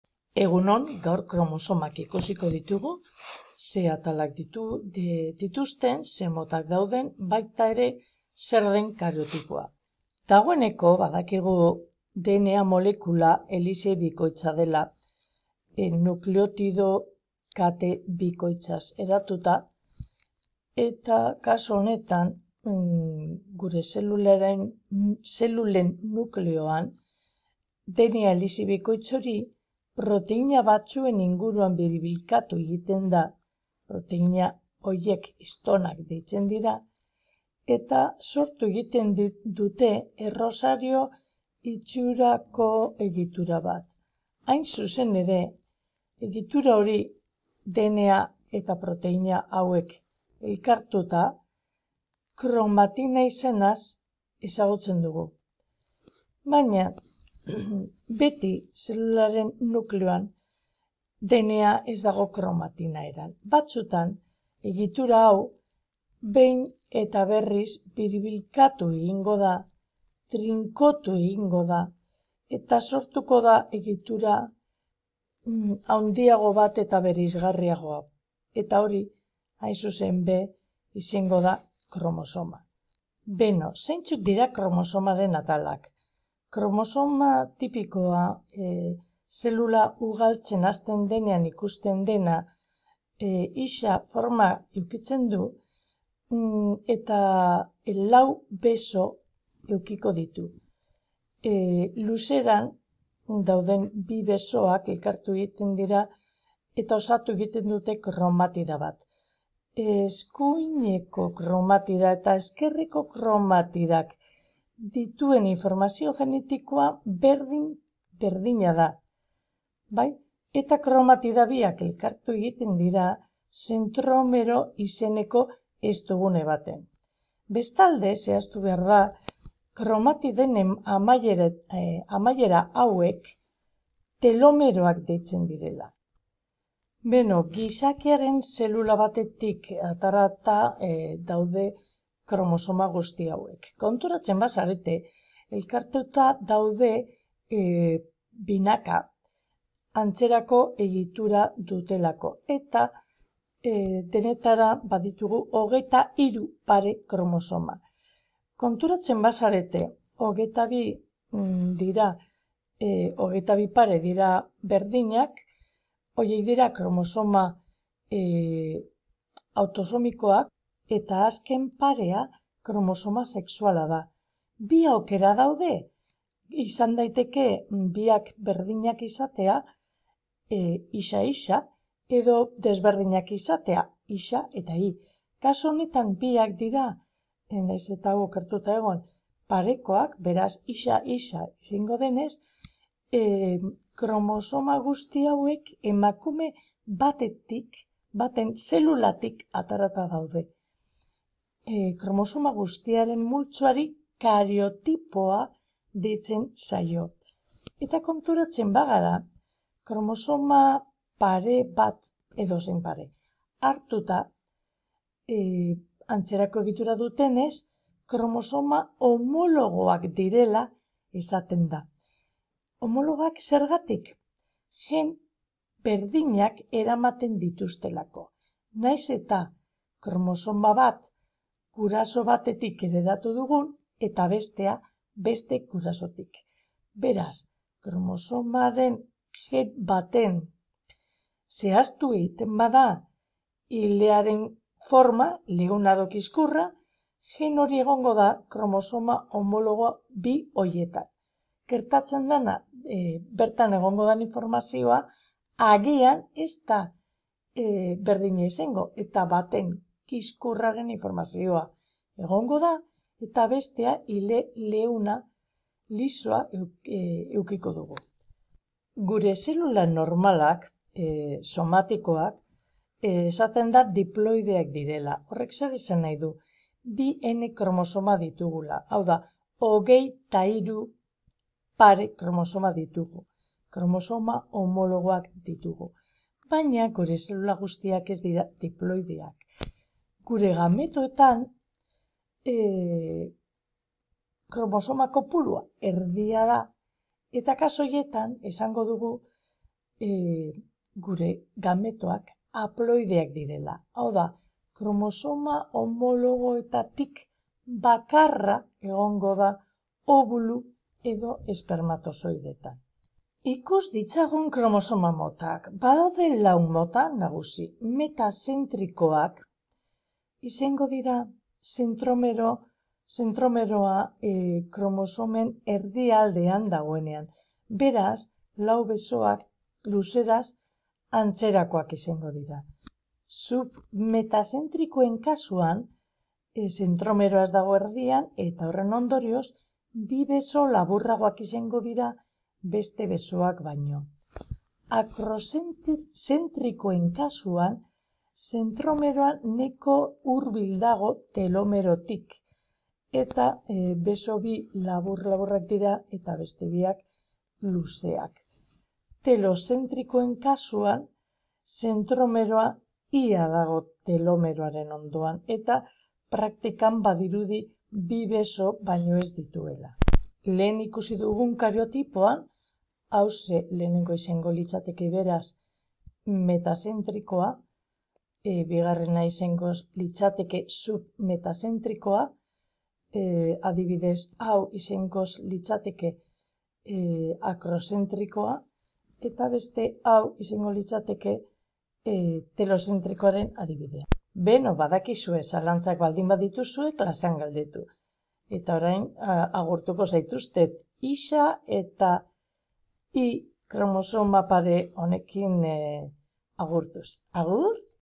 Kromosomak
Nola sortzen da kromosoma? Zeintzuk dira bere atalak? Zenbat mota daude? Zer da kariotipoa? Bideo honetan saiatzen gara itaun horiek erantzuten.